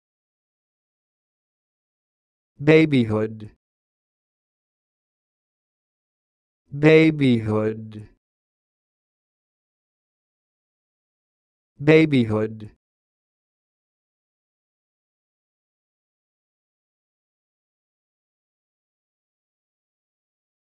Learn how to say this word: "Babyhood"
This is the definition of "Babyhood":
n.  The state or period of infancy.